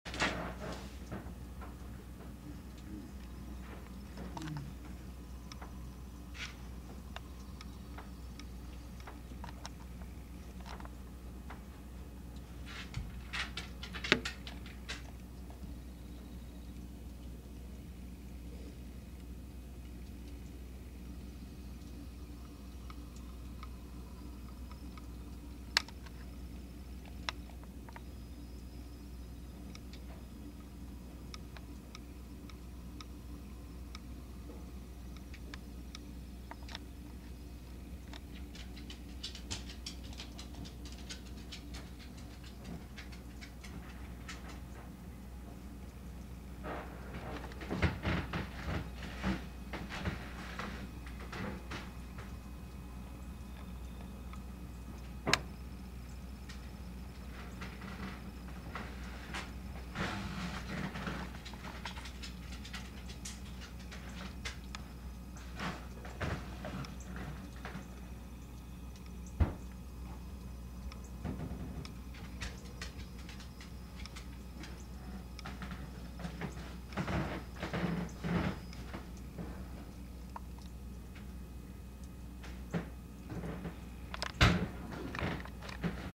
Tour of Backyard During Snowstorm
Snow turns our property into a winter wonderland